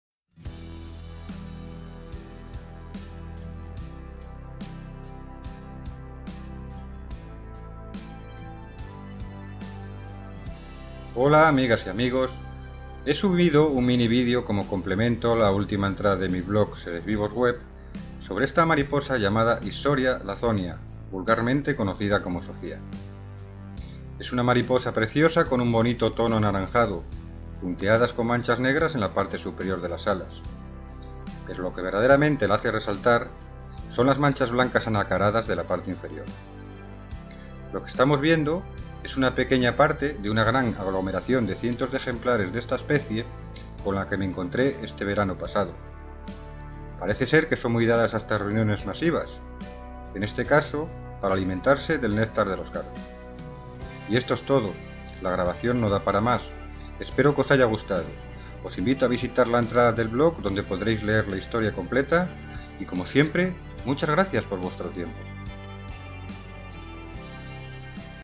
Issoria lathonia (Linnaeus, 1758) - SOFÍA - Concentración alimenticia.
Concentración de un gran número de ejemplares de Ia mariposa Issoria lathonia (Linnaeus, 1758), vulgarmente conocida como Sofía.
El vídeo es complemento de la entrada de mi blog Seresvivosweb, podéis visitarla en esta dirección